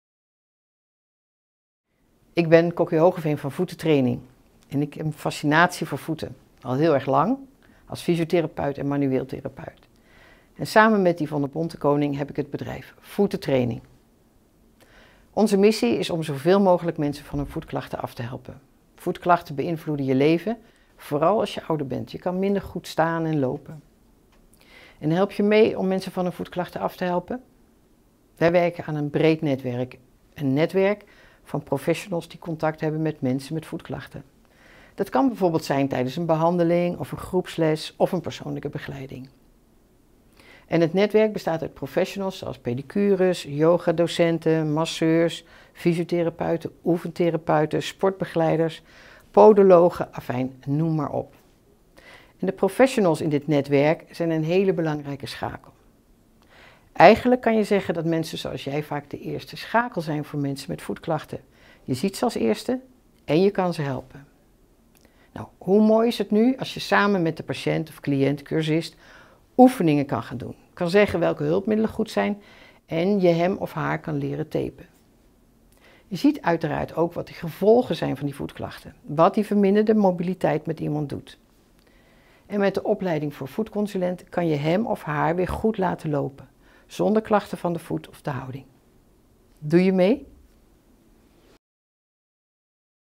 Voetoefeningen voor alle professionals in zorg, welzijn en sport.
Help je cliënten met voetoefeningen! Voetentraining presenteert een compacte opleiding voor alle professionals in zorg, welzijn en sport. Met handige video's en pdf;s voor je cliënten, twee praktijkdagen en een e-learning met info over alle voetklachten. Kijk op wijvoetentrainers.nl